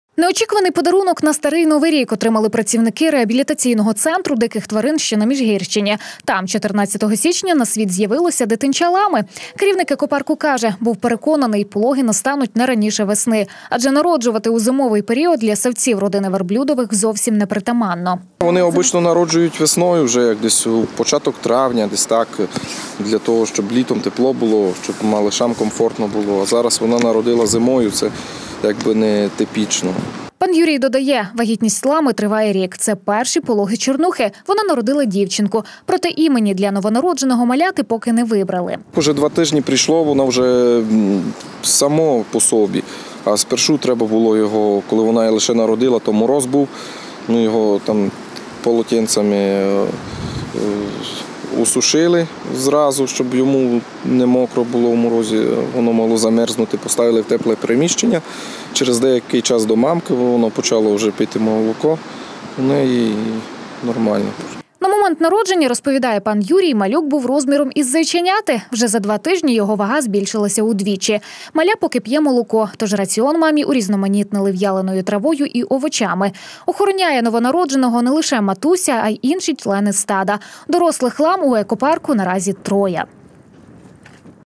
Неочікуване поповнення
Дитинча лами з'явилося на світ в Екопарку на Міжігірщині. Такого поповнення господарі центру ніяк не очікували, адже зазвичай ссавці родини верблюдових народжуються у весняний та літній періоди.